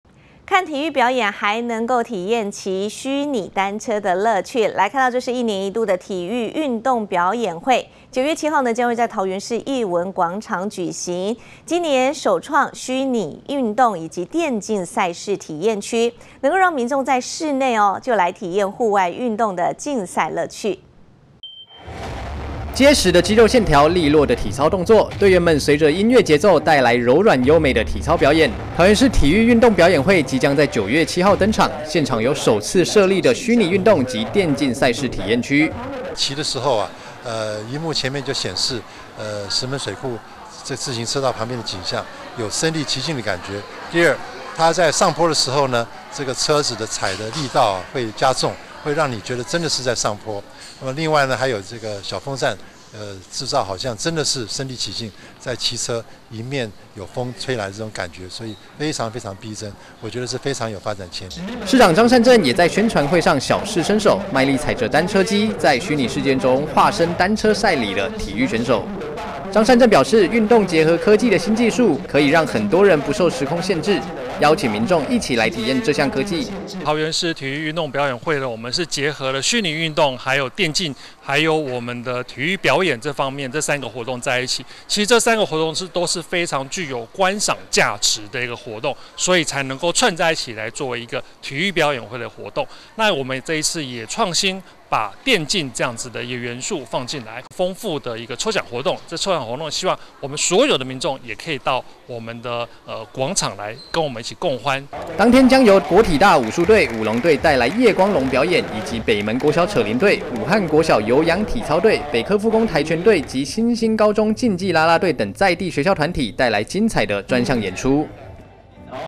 體表會9/7登場 首設虛擬運動.電競區│T-NEWS聯播網
看體育表演，還能體驗騎虛擬單車的樂趣！一年一度的體育運動表演會9/7將在桃園市藝文廣場舉行，今年首創虛擬運動及電競賽事體驗區，能讓民眾在室內體驗戶外運動的競賽樂趣。